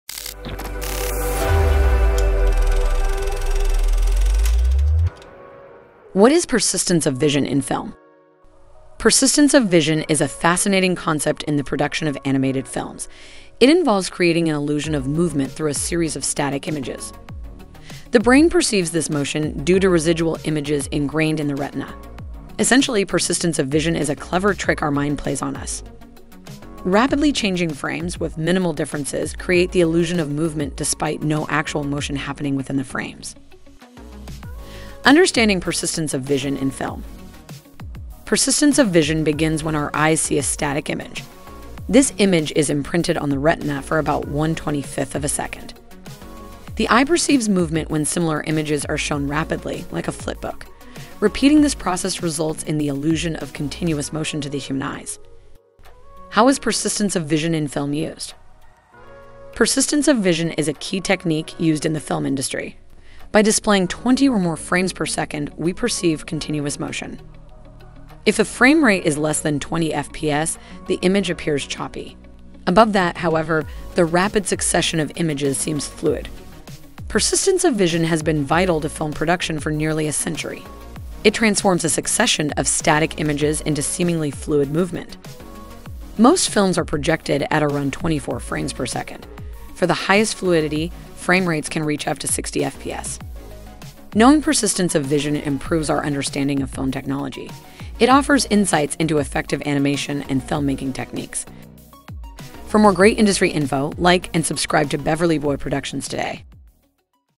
WHAT IS PERSISTENCE OF VISION IN FILM?
Uncover the magical phenomenon of 'Persistence of Vision in Film' with us. This video dives into the complex science behind the illusion of movement that has revolutionized the world of animated films. Learn how our brain and eyes team up to perceive static images as seamless motion, giving life to characters on screen.

We start by explaining the basic concept of persistence of vision in film, then delve into its fascinating mechanism. You'll understand how an image, briefly imprinted on the retina, tricks our brain into seeing continuous motion when similar images are shown in quick succession.

Further, we explore how this intriguing principle is applied in the film industry to create engaging and fluid animations. Discover how the frame rate plays a pivotal role in this process, as anything above 20 frames per second results in a smooth visual experience for the audience.

To wrap it up, we look at the historical aspect and current trends, noting that most films today are projected at around 24 frames per second for optimum fluidity. High-end productions may even reach up to 60 fps for the most seamless viewing experience.

Join us on this enlightening journey to unravel the magic behind the moving images we love on screen. This is a must-watch for any film enthusiast or aspiring animator looking to understand the fundamental principles of animated filmmaking.

0:00 WHAT IS PERSISTENCE OF VISION IN FILM?
0:34 UNDERSTANDING PERSISTENCE OF VISION IN FILM
0:55 HOW IS PERSISTENCE OF VISION IN FILM USED?
1:42 SUBSCRIBE TO BEVERLY BOY PRODUCTIONS